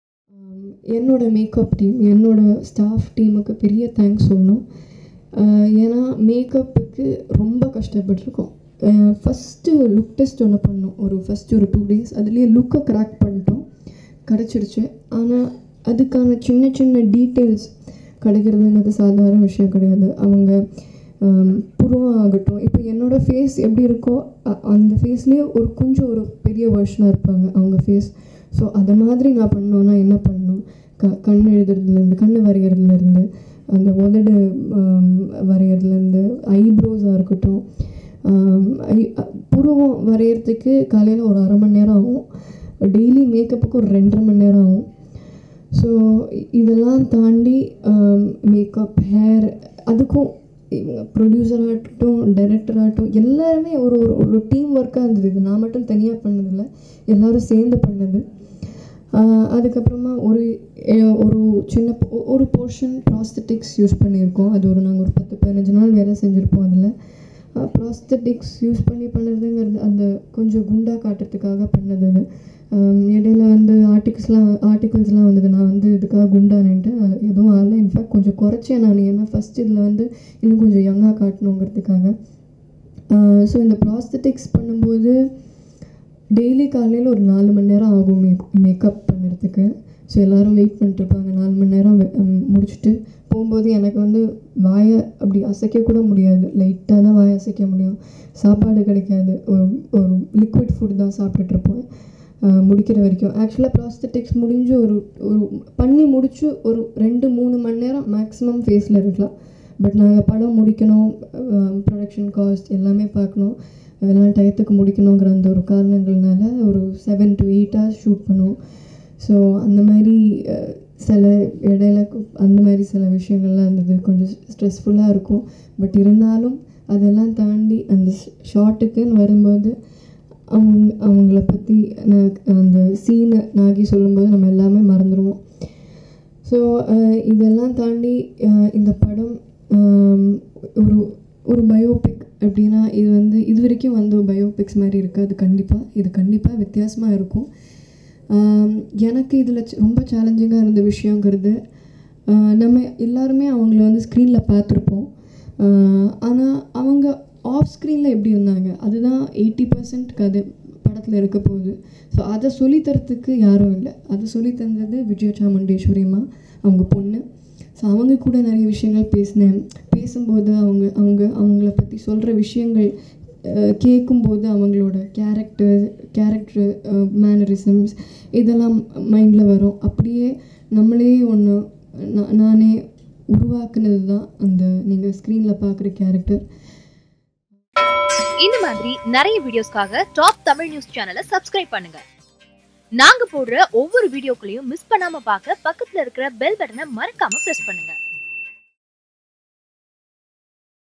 4 மணிநேரம் மேக்கப், 8 மணிநேரம் ஷூட்டிங் | Keerthy Suresh | Nadigayar Thilagam
நடிகையர் திலகத்திற்காக தினமும் 4 மணி நேரம் மேக்கப் ; 8 மணி நேரம் ஷூட்டிங்: கீர்த்தி சுரேஷ் பேச்சு